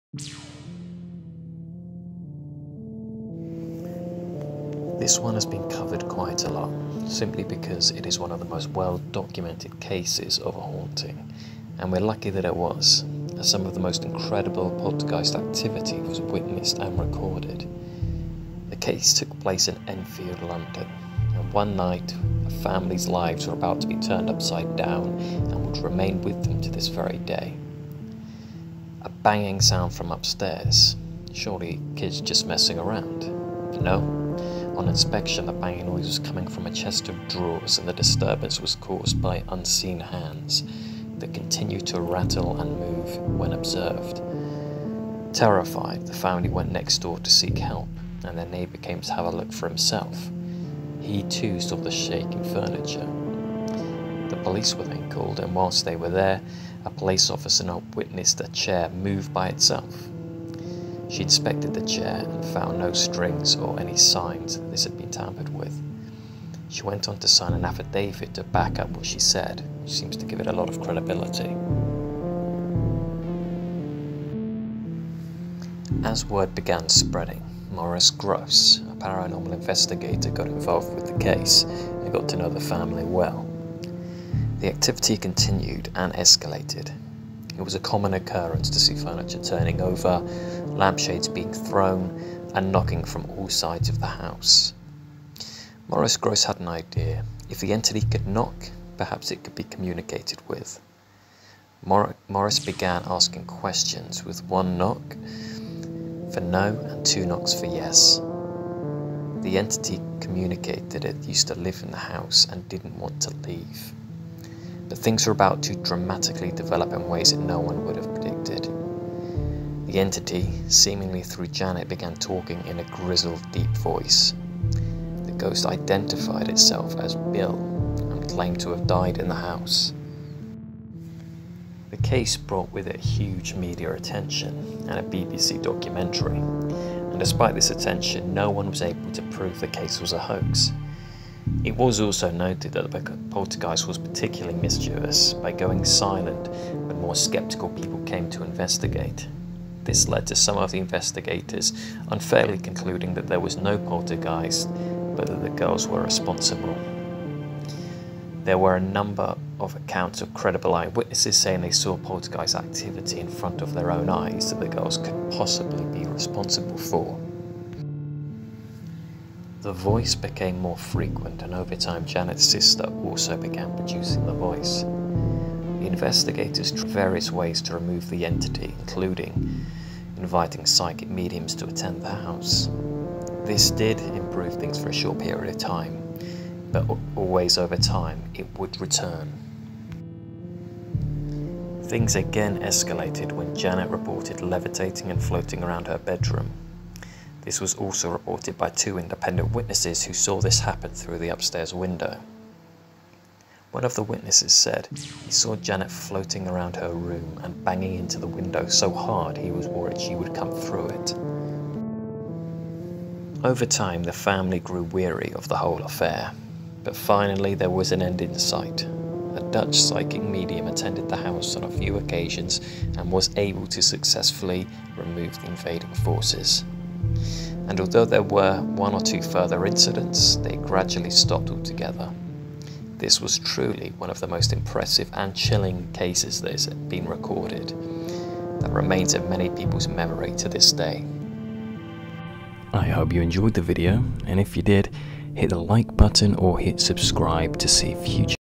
The Enfield Poltergeist
Possibly the most documented poltergeist case of all time.